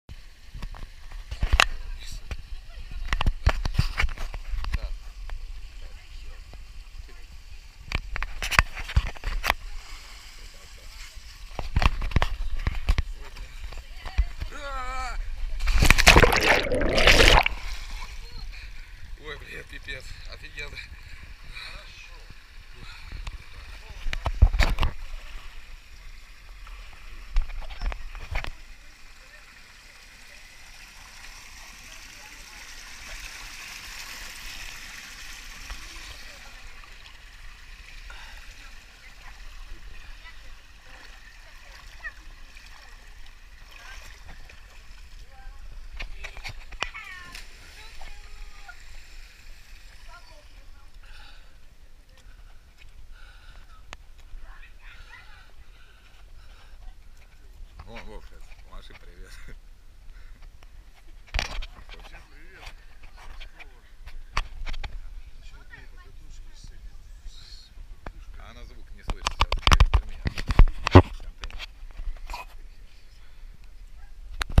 Купание в роднике
Прыжок в очень холодную воду.